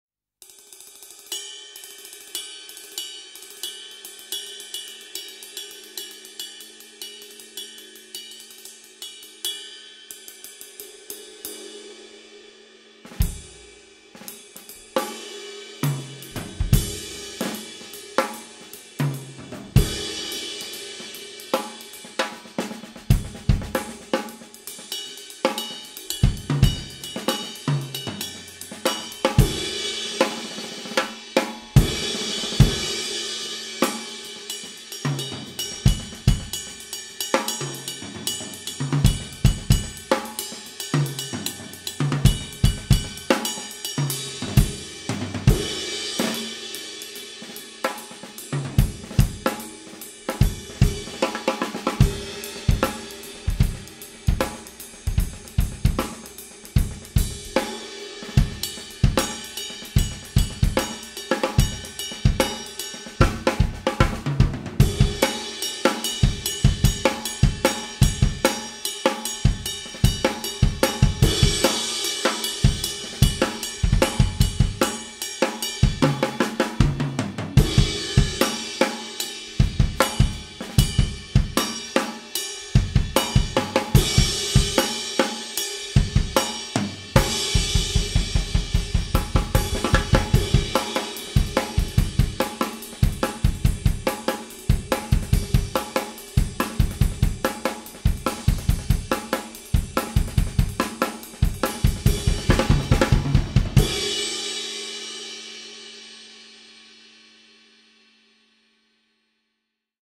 Paiste 22" 2002 Ride Cymbal - 3218g (1061622-1032520GG)
Paiste 22" 2002 Ride Cymbal

Other gear used in this video:
• Cymbals (From drummer's L to R): Paiste 17" Signature Steve Jordan Style Hi-Hats, Paiste 20" Formula 602 Heavy Crash Cymbal